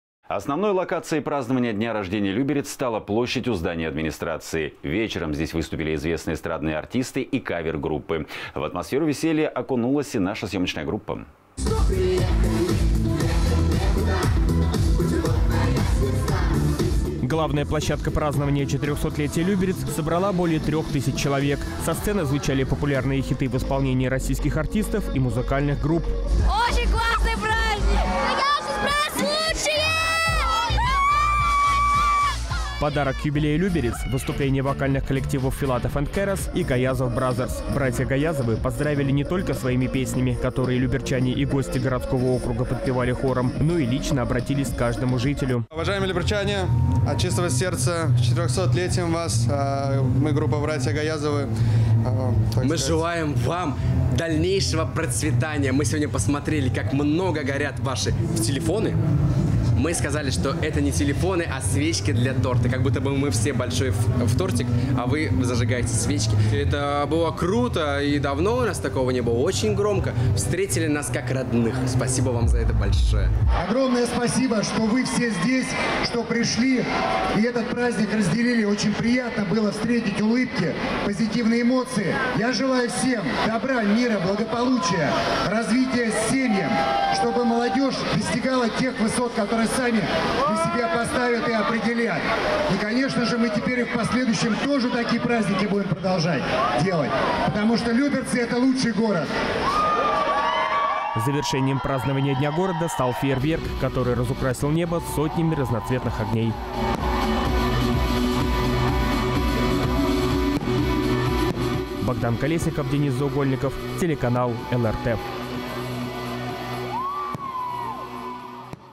Праздничный концерт и салют в честь юбилея Люберец
Основной локацией празднования дня рождения Люберец стала площадь у здания администрации. Вечером здесь выступили известные эстрадные артисты и кавер-группы. В атмосферу веселья окунулась и наша съёмочная группа.

Главная площадка празднования 400-летия Люберец собрала более трёх тысяч человек. Со сцены звучали популярные хиты в исполнении российских артистов и музыкальных групп.

Подарок к юбилею Люберец – выступление вокальных коллективов «Filatov & Karas» и «Gayazovs Brothers». Братья Гаязовы поздравили не только своими песнями, которые люберчане и гости городского округа подпевали хором, но и лично обратились к каждому жителю.

Тимур и Ильяс Гаязовы, солисты группы «Gayazovs Brothers»: «Уважаемые люберчане, от чистого сердца с 400-летием вас. Мы братья Гаязовы. – Мы желаем вам дальнейшего процветания. Мы сегодня посмотрели, как много горят ваши телефоны. Мы сказали, что это не телефоны а свечки для торта. Как будто мы все большой тортик, а вы зажигаете свечки. Это было круто. Давно у нас такого не было. Очень громко! Встретили нас, как родных. Спасибо вам за это большое».

Владимир Волков, глава г.о. Люберцы: «Огромное спасибо, что вы все здесь, что пришли и этот праздник разделили. Очень приятно было встретить улыбки, позитивные эмоции. Я желаю всем добра, мира, благополучия, развития семьям. Чтобы молодёжь достигала тех высот, которые сама для себя поставит и определит. И, конечно же, мы теперь и в последующем тоже такие праздники будем продолжать делать, потому что Люберцы – это лучший город».

Завершением празднования Дня города стал фейерверк, который разукрасил небо сотнями разноцветных огней.